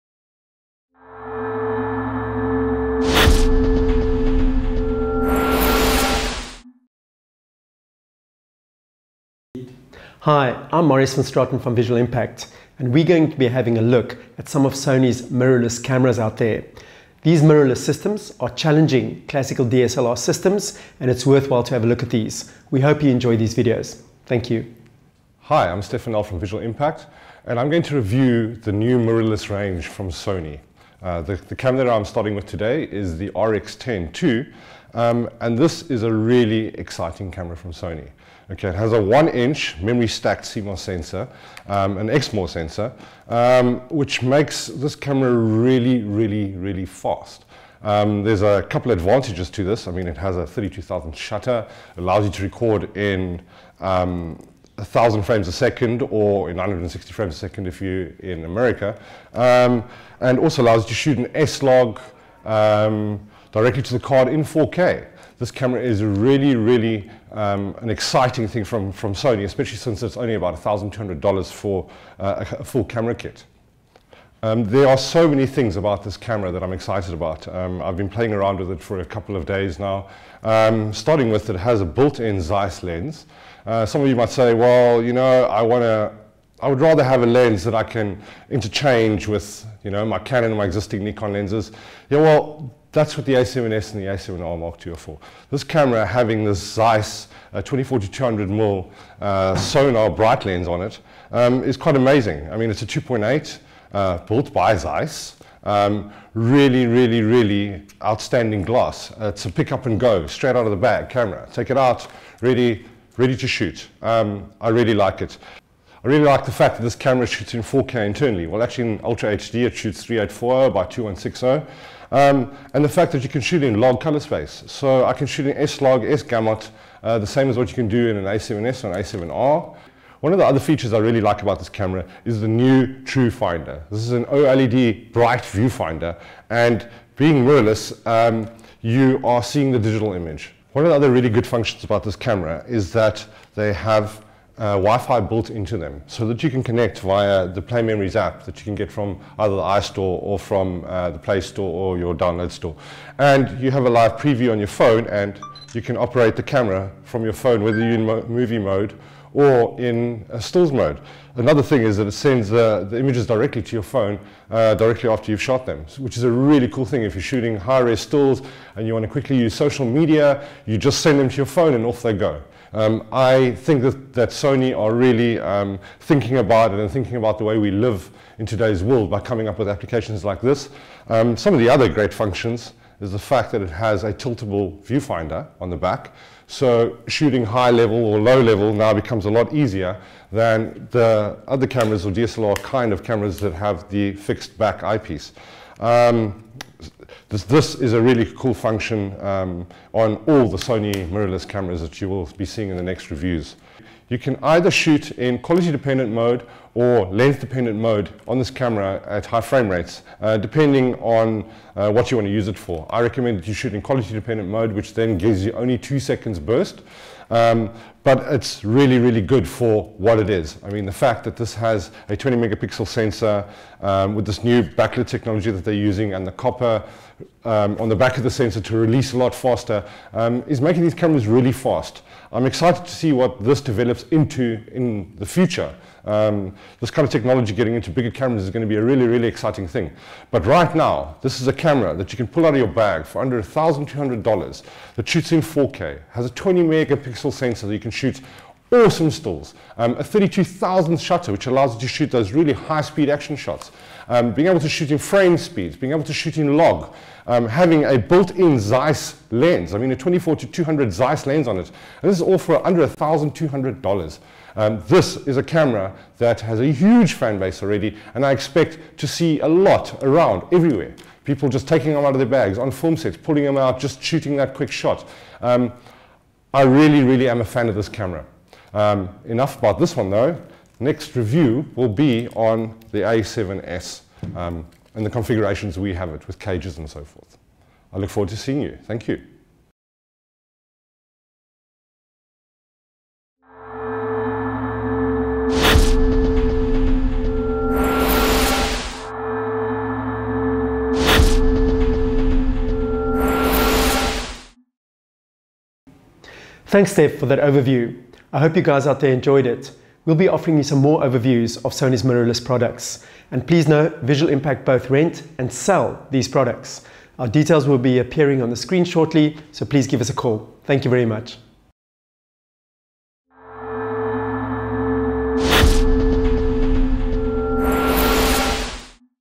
Sony Alpha RX10ii
In this video we overview the Sony Alpha RX10 ii mirrorless camera.

Sony's latest cameras are leaving some of the competition behind and we see the demand in sales and rentals.

Have a look at some slow motion examples on our slow motion play list.

The 1 inch sensor has a 20-megapixel resolution and offers high frame rates and 4K recording.
The faster sensor enables 960 frames per second shooting for 2 seconds, which allows you to produce slow-motion video clips.
One can also shoot 240fps and 480fps for longer durations as well as 14 fps continuous-shooting speeds with fixed exposure.
The camera can record 4K to an SD card using the XAVC S 100Mbps codec for as long as 29 minutes per clip.
It can pre-record or buffer frames so that it doesn't matter if you just missed the decisive moment as long as you were framed up.
The electronic shutter goes up to 1/32 000 second which is one stop  above the 1/16 000 second most cameras in this class support. This is useful to close down that additional stop in bright sunlight.